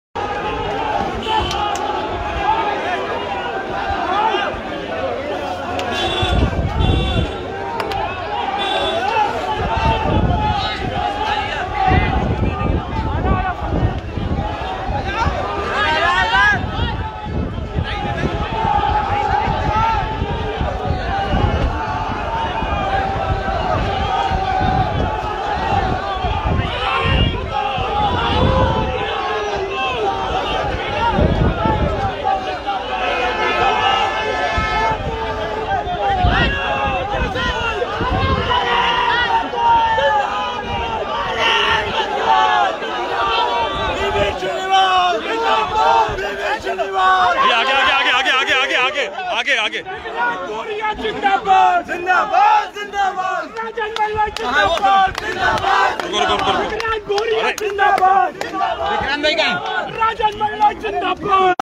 Youth Congress कांग्रेस के राष्ट्रीय अध्यक्ष श्रीनिवास बी वी जी
भारतीय युव कांग्रेस के राष्ट्रीय अध्यक्ष श्रीनिवास बी वी जी का झीलों की नगरी भोपाल में मध्यप्रदेश युवा कांग्रेस द्वारा एयरपोर्ट आगमन पर भव्य स्वागत।
Srinivas BV